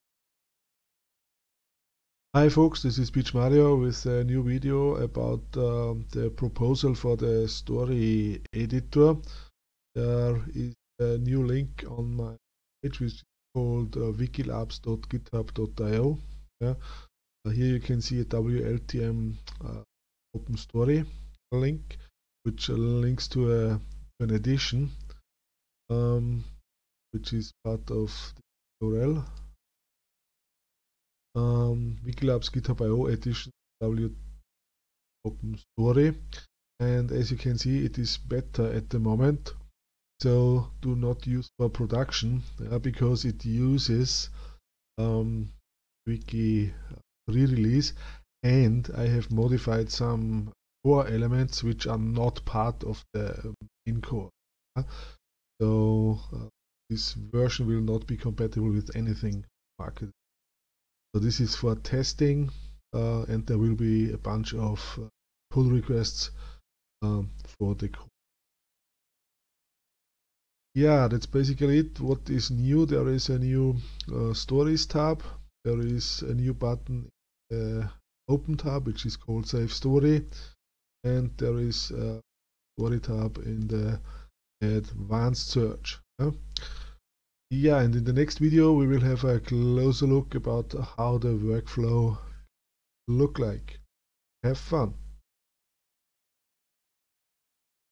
Story Editor for TiddlyWiki
A short intro about a proposed Story Editor for TiddlyWiki.